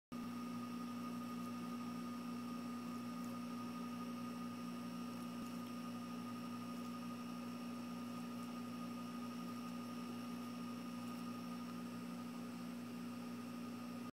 Dry nose
Testing the video on my new phone by recording tired Lola who apparently has a dry nose lol